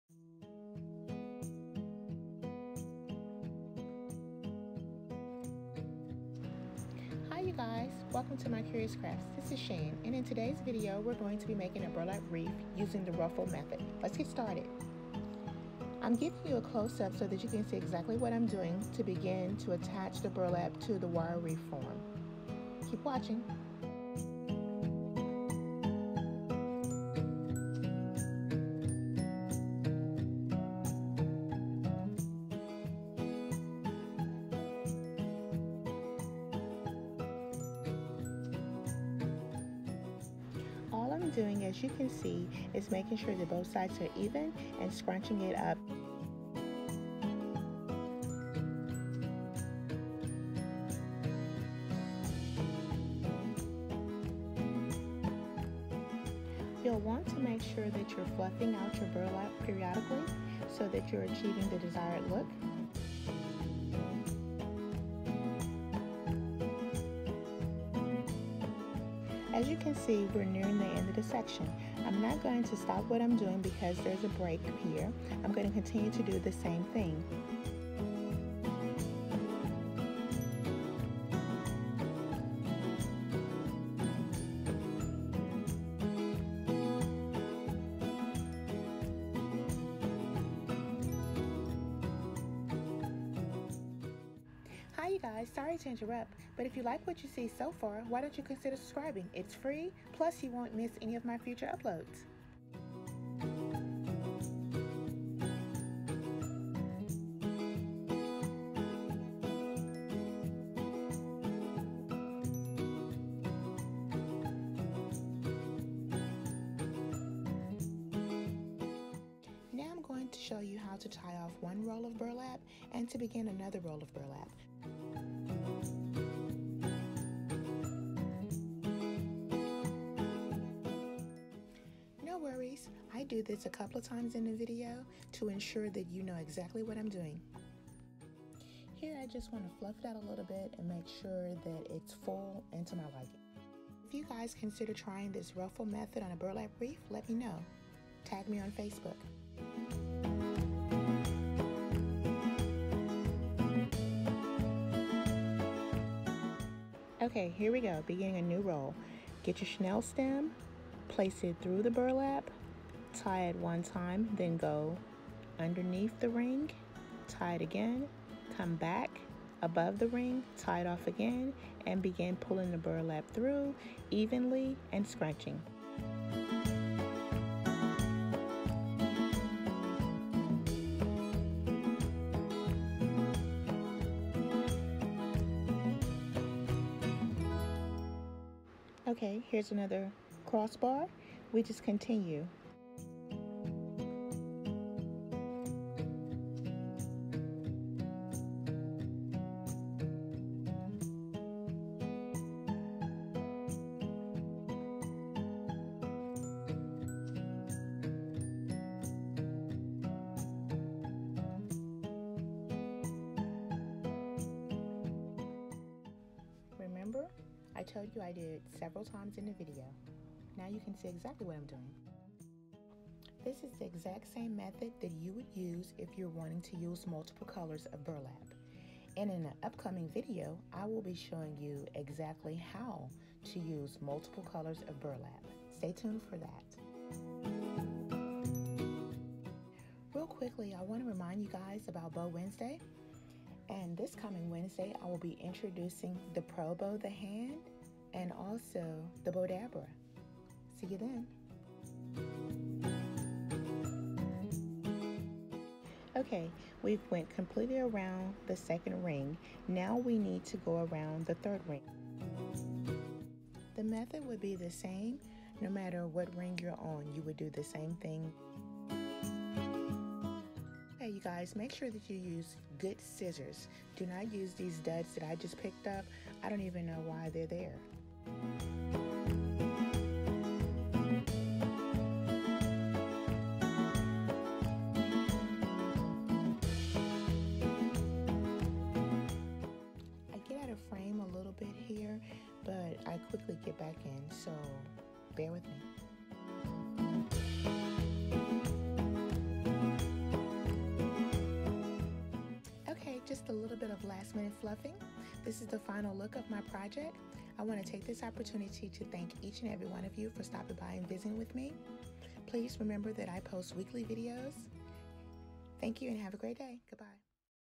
How To Make A Burlap Wreath || Burlap Wreath Tutorial || Ruffle Method {{2019}}
Hello beautiful people! In today's video, I will be showing you how to make a burlap wreath utilizing the ruffle method. This burlap wreath tutorial is great for beginners. This burlap wreath can be for any season, although I'm using Fall colors, you are able to use any colors, it's your choice. Have an amazing week!
Hugs,
~Shan